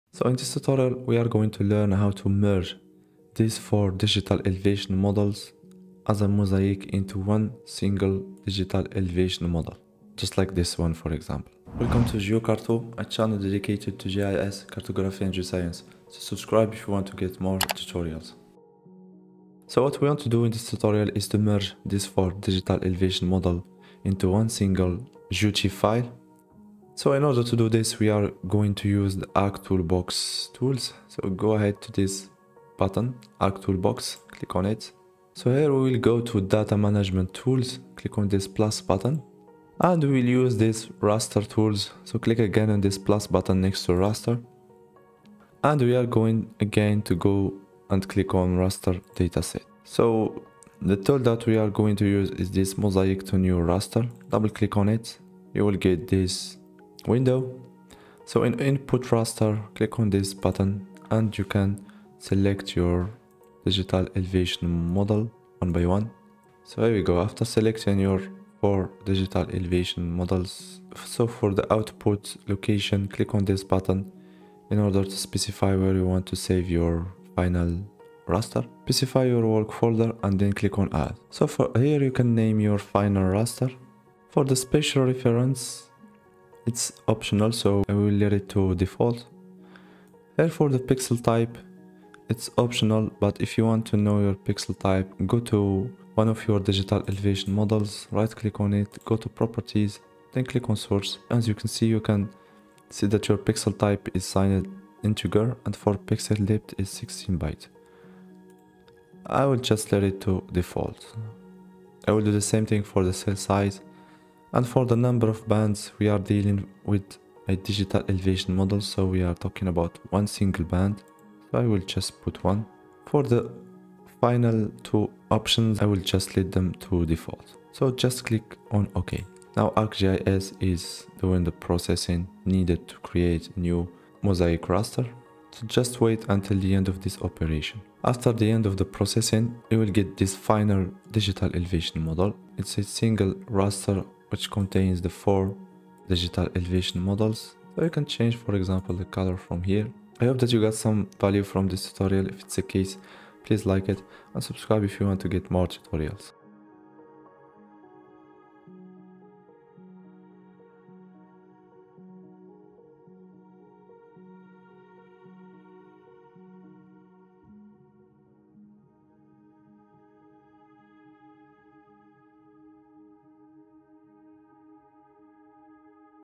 Merge raster DEM in ArcGIS
This video is a step-by-step tutorial on how to merge multiple raster DEM (Digital Elevation Model) into one single raster file in ArcGIS.